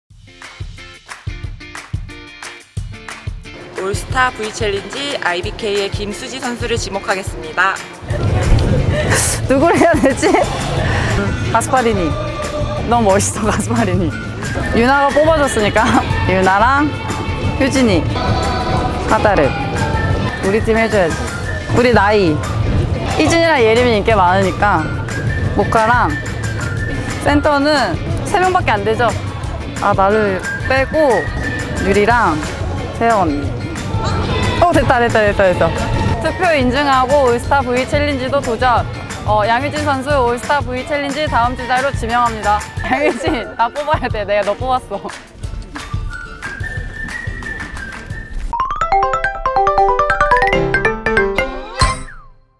[코보티비] 국대 센터들끼리 주고받는(?) 올스타V챌린지!
배유나는 김수지 지목 김수지는 양효진 지목ㅋㅋㅋㅋ